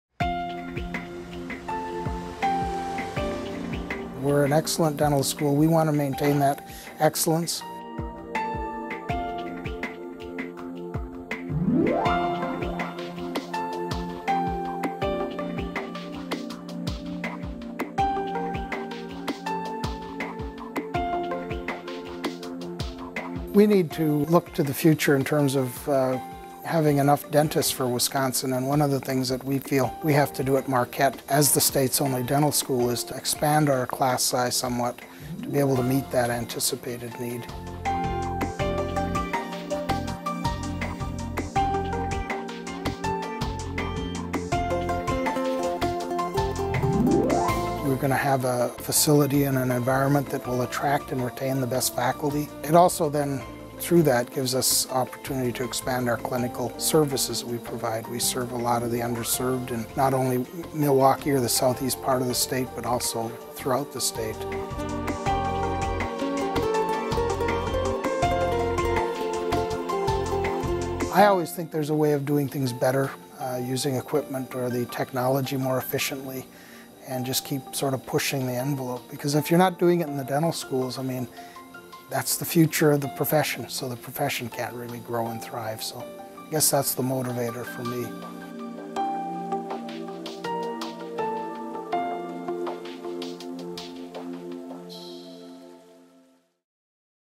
Marquette University School of Dentistry — Building for the Future
Funding for an expansion to the Marquette University School of Dentistry was included in Gov. Scott Walker's capital budget, and the recommendation to include it was approved March 16, 2011, by the State of Wisconsin Building Commission. The School of Dentistry is requesting
state funds toward the construction of a 40,000-square-foot expansion of its current facility.

As Wisconsin's only dental school, Marquette offers the most advanced clinical resources and instructional technology available, supporting a dramatically different, one-of-a kind curriculum developed by our faculty to better prepare students for dental practice and patient care. Marquette's dental students provide high-quality oral health care for more than 26,000 patients — many on Medicaid or uninsured — with 91,000 visits at seven statewide clinics annually.